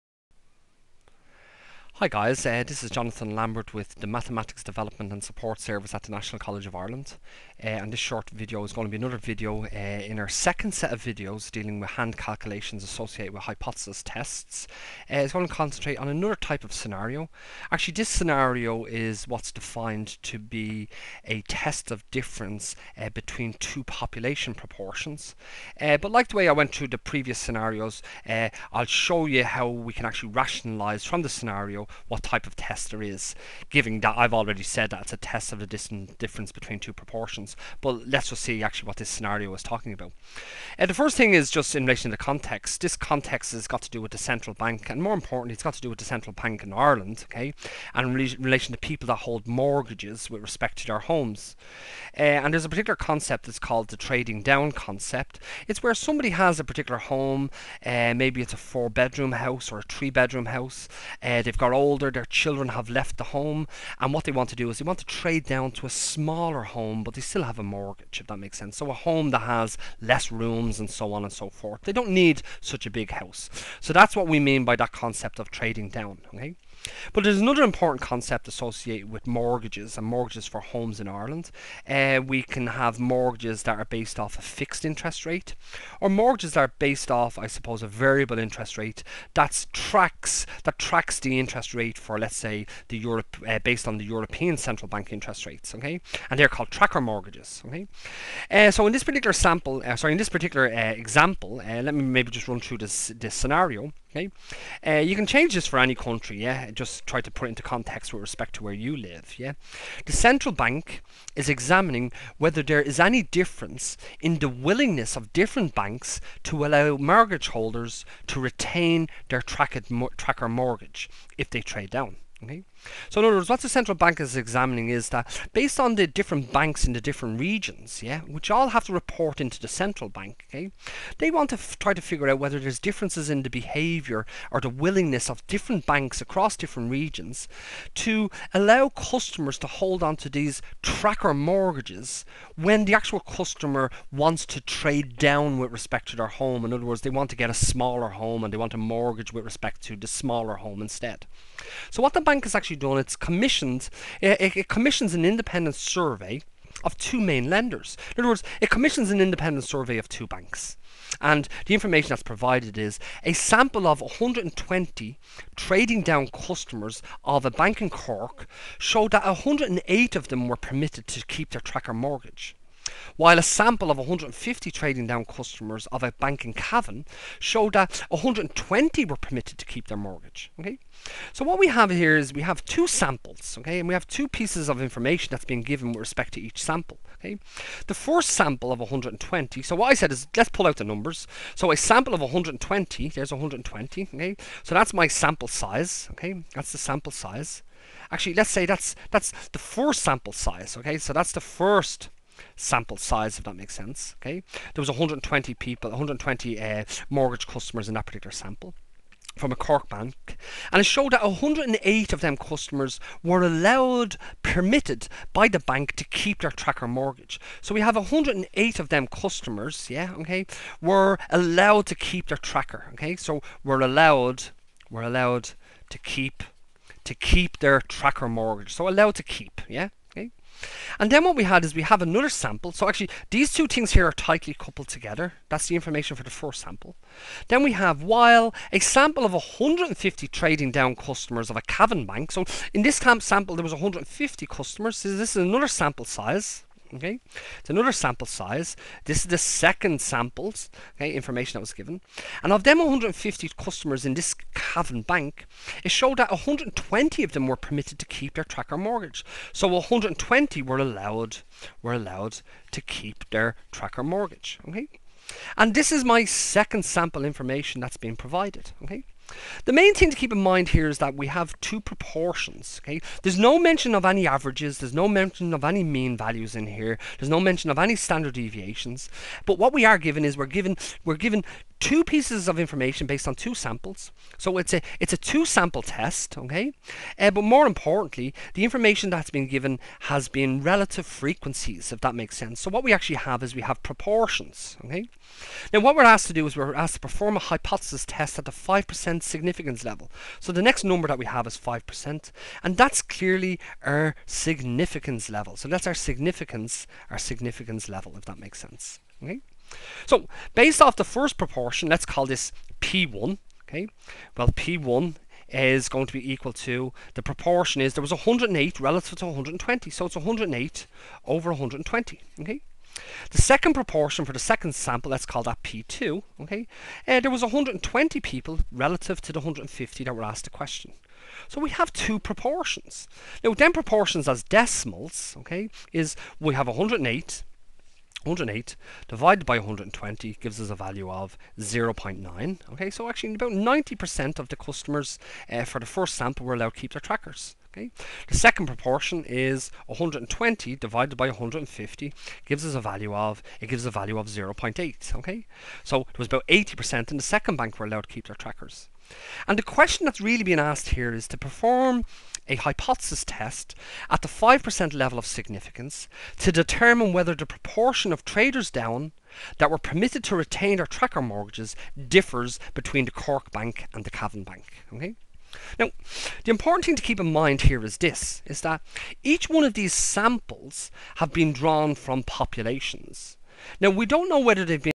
Hypothesis Testing by Hand: zTest of the Difference Between Two Population Proportions - Part 1
This short video presents a hand calculation of a Hypothesis Test for the difference between two Population Proportions. The presentation relies upon the z-statistic associated with the difference between two proportions.